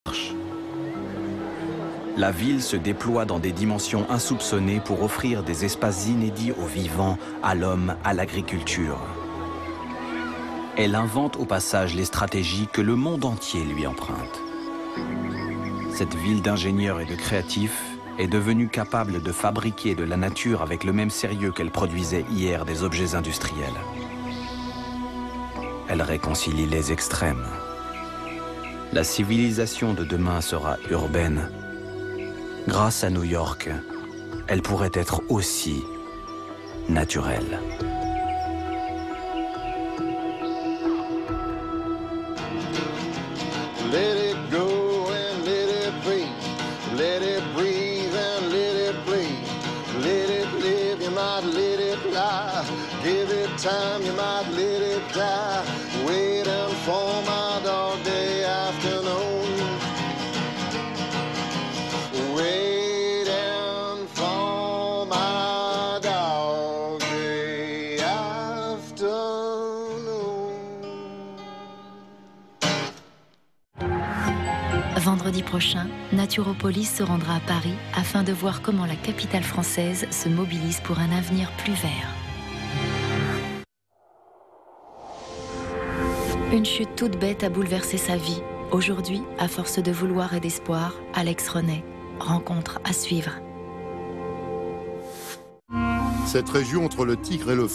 La transformation verte de New York (Fin)
Reportage sur New York et sa végétalisation.
Nature O Polis